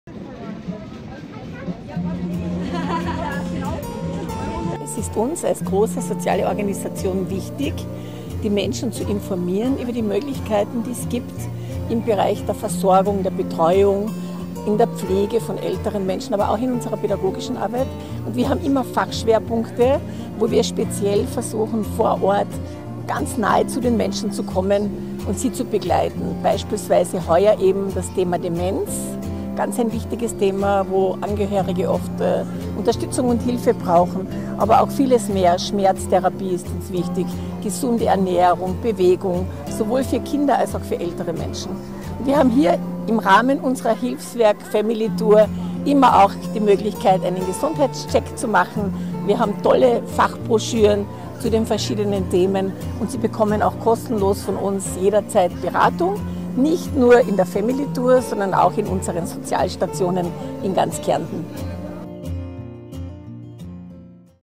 Hilfswerk On Tour
Hilfswerk on Tour. Präsidentin Elisabeth Scheucher und die Expertinnen des Hilfswerks informierten über Pflege und Betreuung, Kinder und Jugend, Gesundheit und vieles mehr! Klagenfurt am 4.9.2019.